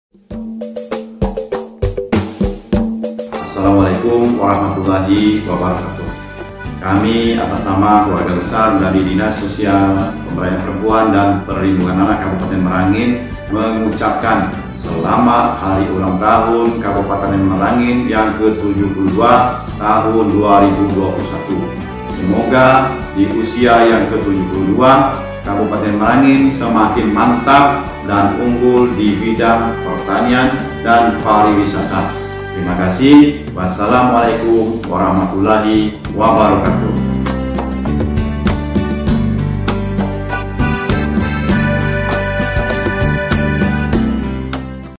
HUT. KAB. MERANGIN KE 72 # drs. ELVIS SURYADINATA
Memperingati hari ulang tahun Merangin yang Ke 72 Oleh Kepala Dinas Sosial PPPA Kab.  Merangin